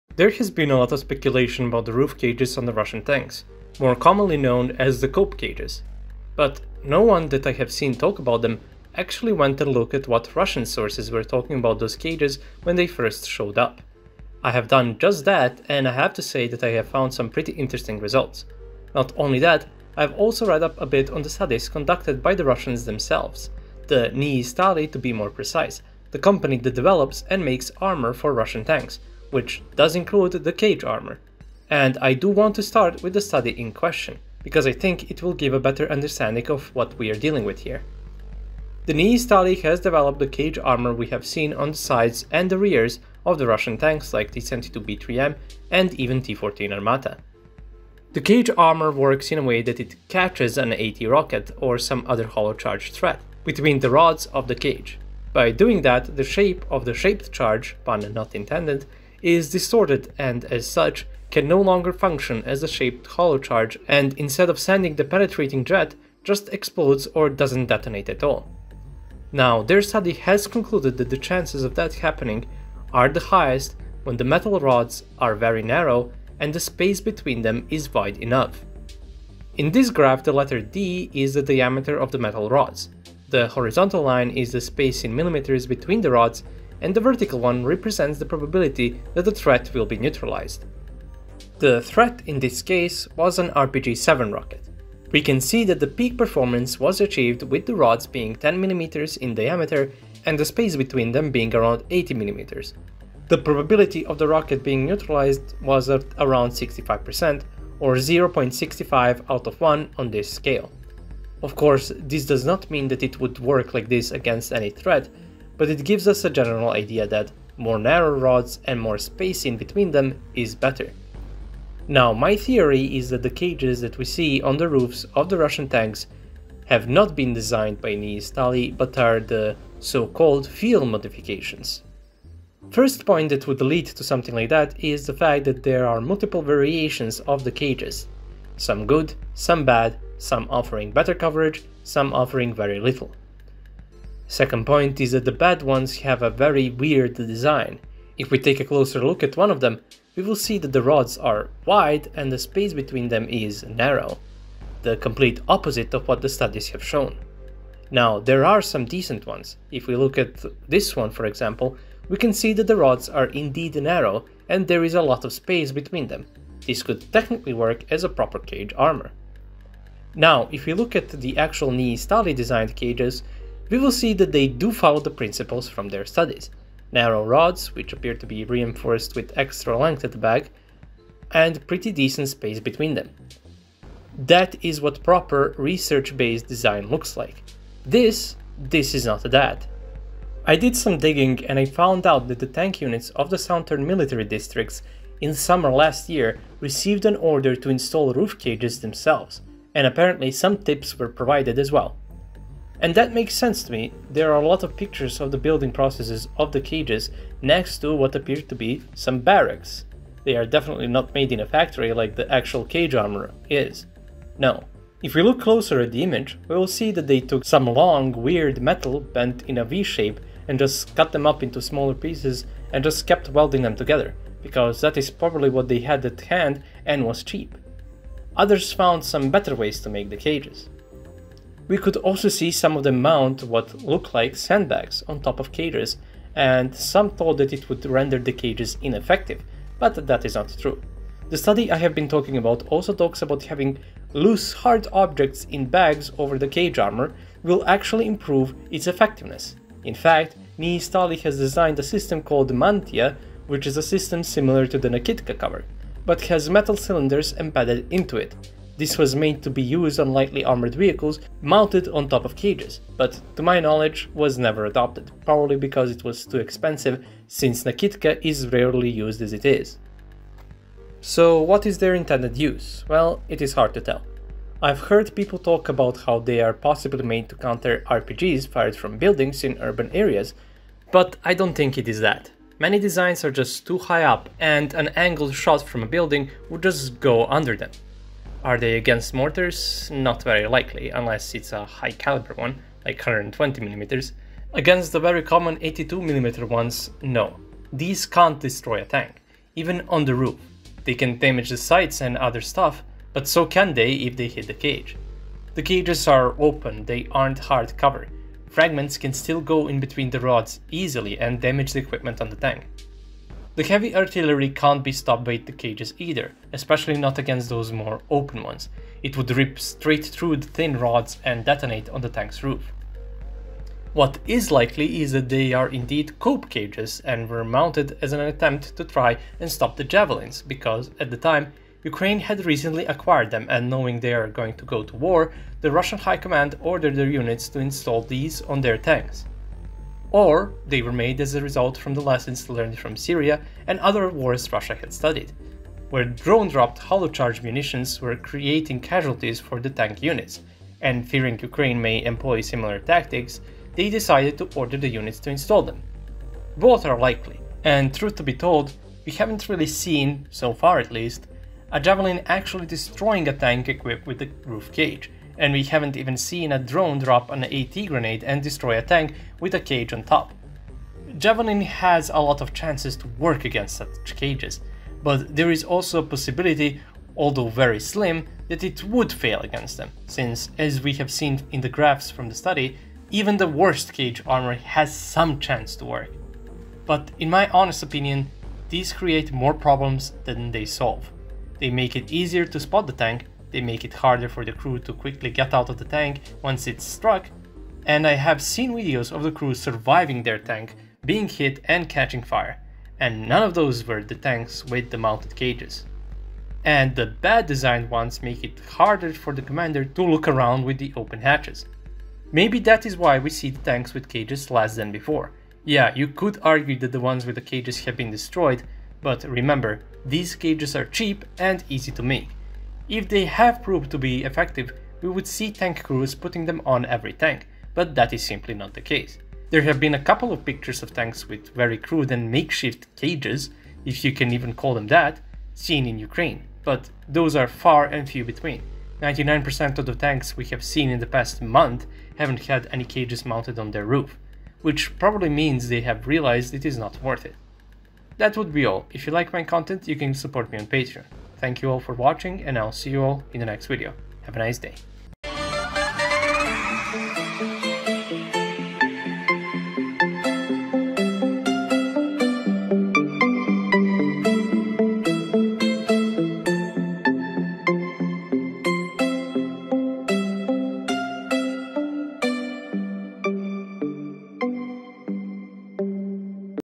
The Truth About COPE CAGES
There has been a lot of speculation about the roof cages on the Russian tanks, more commonly known as the "Cope Cages".